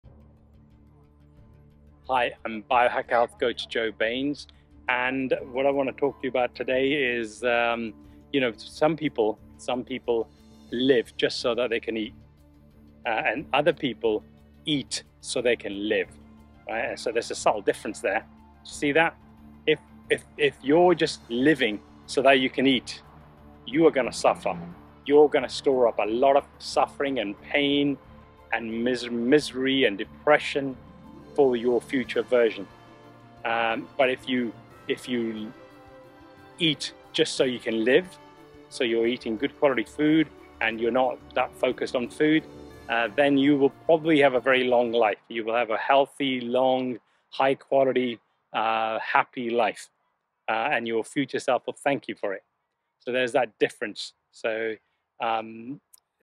Living to eat or eating to live, Which is better?
Summary
Living to eat can lead to suffering and negative consequences, while eating to live promotes a healthy and happy life.

Highlights
😔 Living solely to eat can result in suffering, pain, and depression.
🍏 Eating for sustenance and health leads to a long and high-quality life.
🙌 Focusing less on food and more on quality food improves overall well-being.